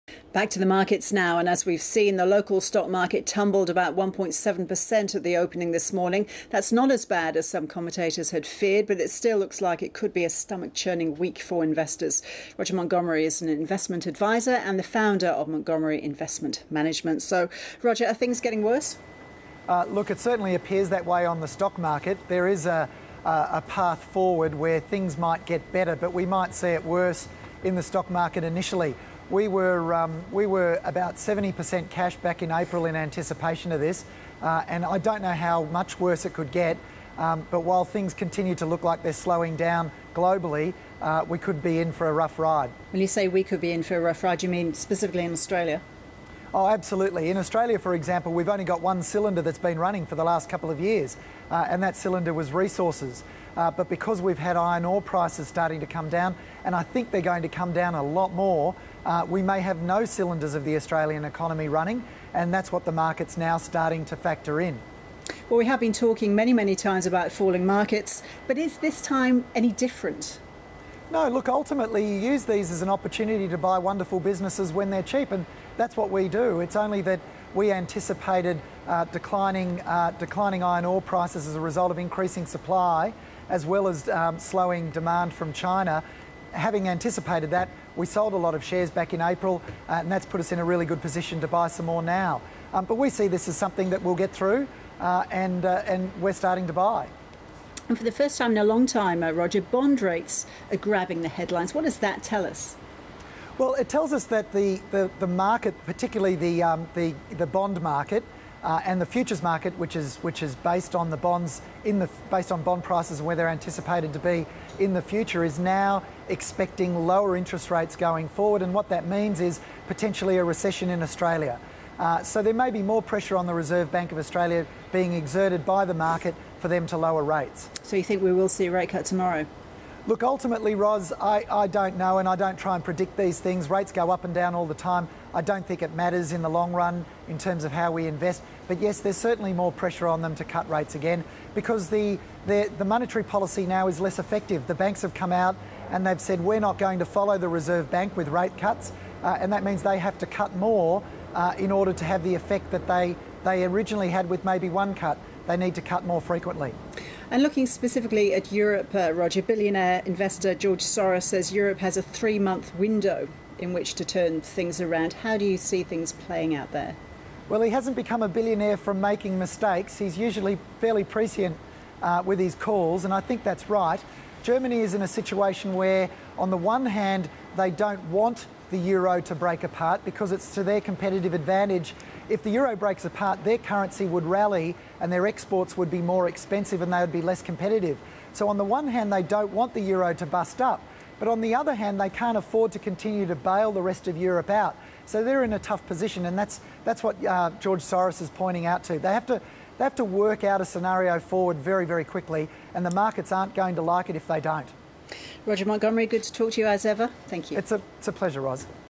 Expert tips further market tumbles
Montgomery Investment Management founder Roger Montgomery believes stockmarket conditions may worsen before improving.